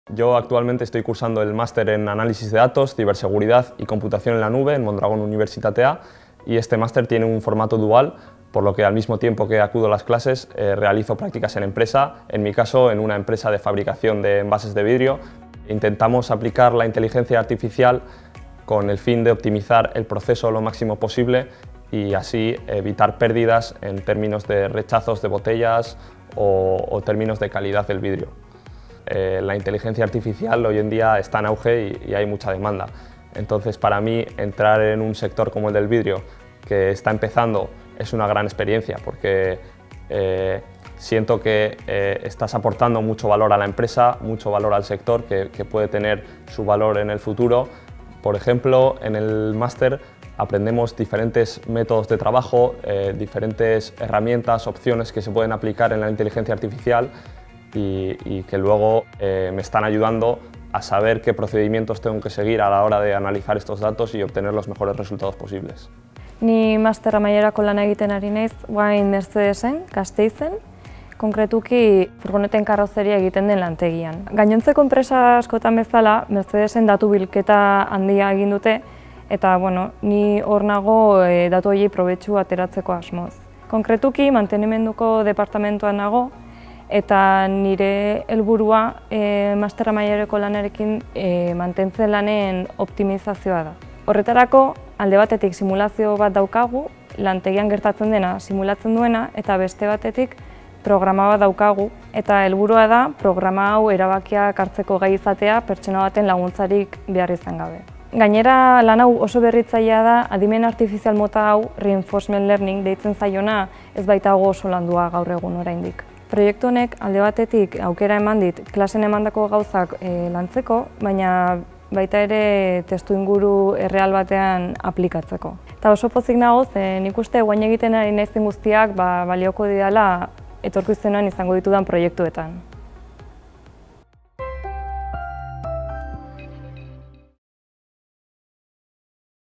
Inteligencia Artificial - Adimen Artifiziala
Dos estudiantes del Máster Universitario en Análisis de Datos, Ciberseguridad y Computación en la Nube nos han explicado cómo están aplicando los conocimientos de Inteligencia Artificial en dos empresas que trabajan en diferentes ámbitos.

Adimen Artifiziala

Datuen Analisia, Zibersegurtasuna eta Hodei-Konputazioan Unibertsitate Masterreko bi ikaslek azaldu digute nola ari diren aplikatzen Adimen Artifizialari lotutako ezagutzak arlo desberdinetan diharduten bi enpresatan.

Artificial Intelligence

Two students from the Master's Degree in Data Analysis, Cybersecurity and Cloud Computing have explained to us how they are applying their Artificial Intelligence knowledge in two companies working in different fields.